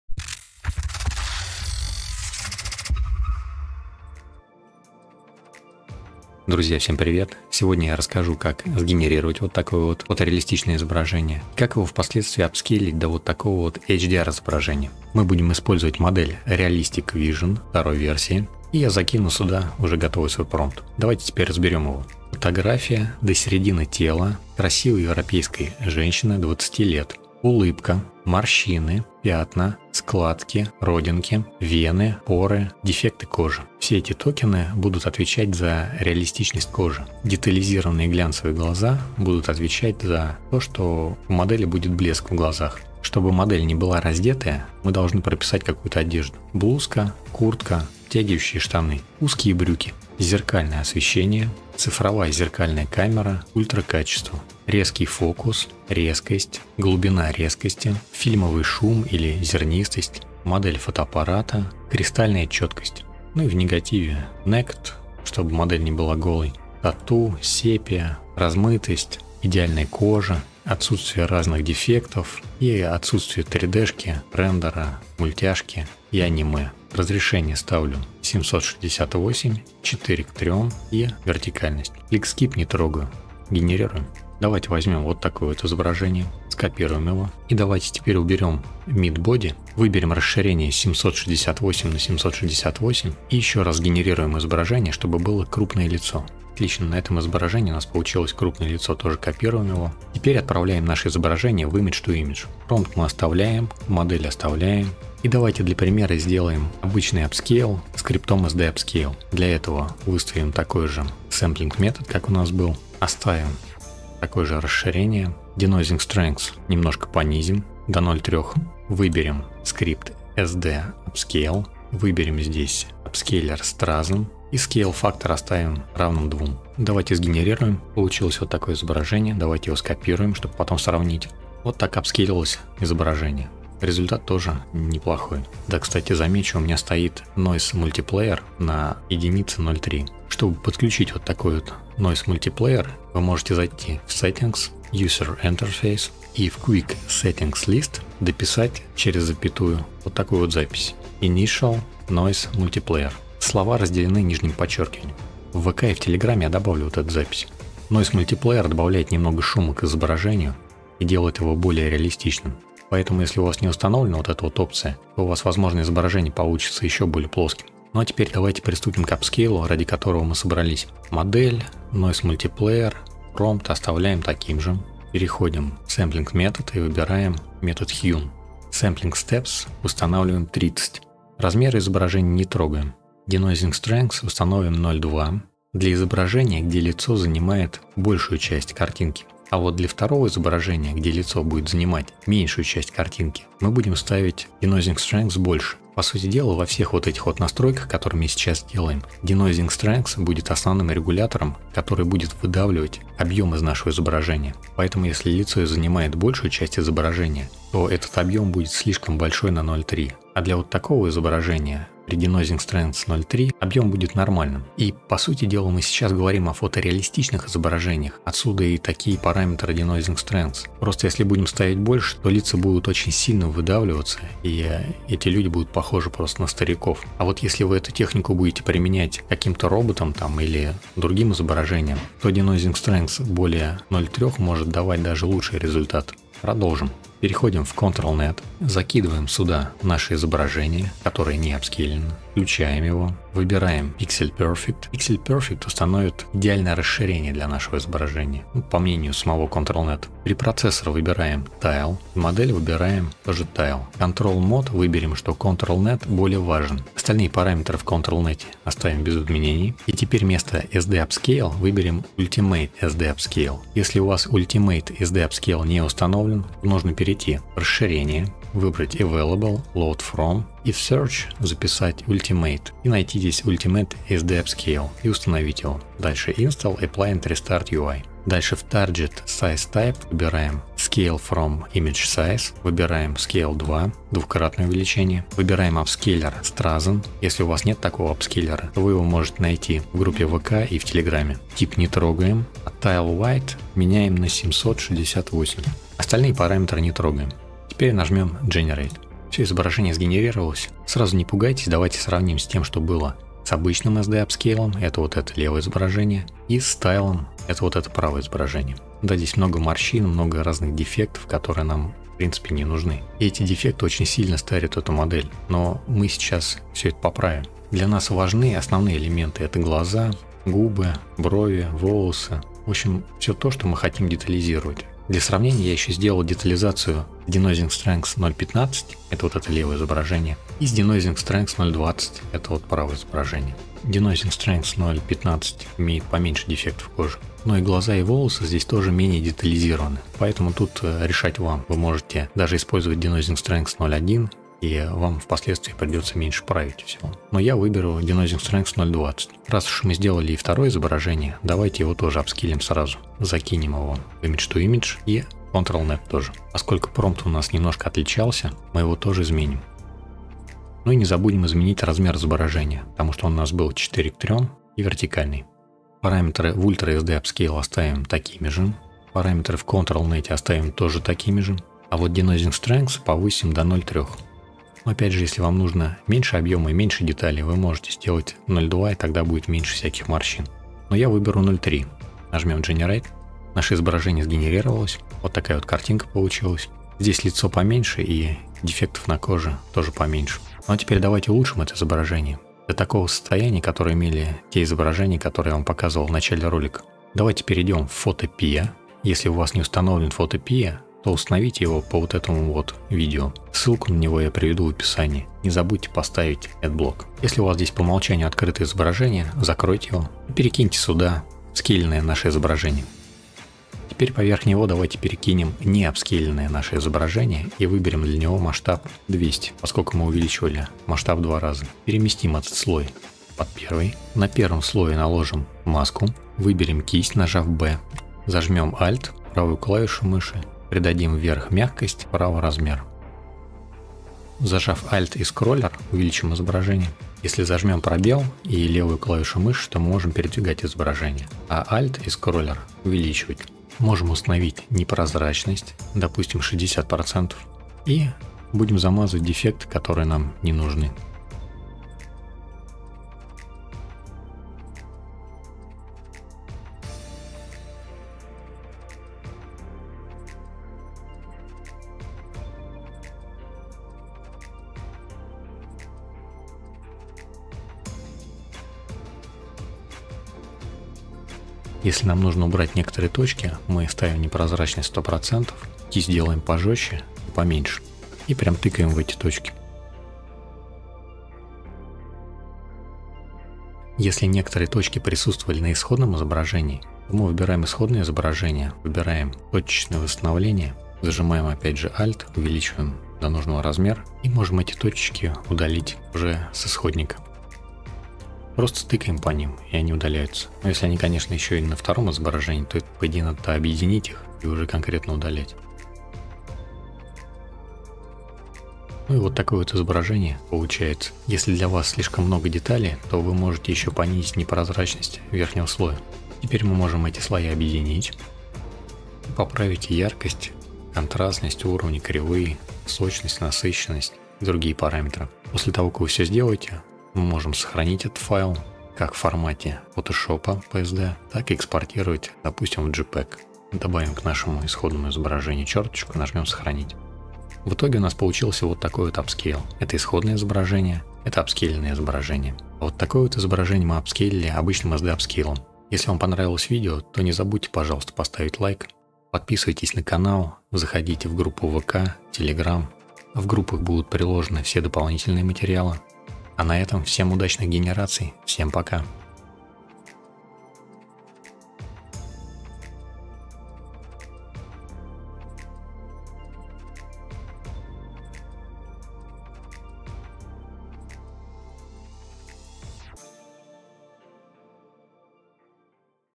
Upscale фотореалистичных изображений | ControlNet Tile | Ultimate SD Upscale | Photopea
Научимся делать Upscale фотореалистичных портретов. Создадим фотореалистичный портрет с текстурированной кожей. Для увеличения изображения будем применять ControlNet Tile. Будем использовать Noise multiplier. Применим Ultimate SD Upscale для получения максимум деталей совместно с ControlNet 1.1 Tile. Вытащим лучшие детали из апскейленого изображения, а полученные дефекты скроем в редакторе Photopea в automatic 1111.

*Все ссылки и дополнительные файлы к проекту в наших группах:*

🍀 *Поддержать автора можно в Boosty*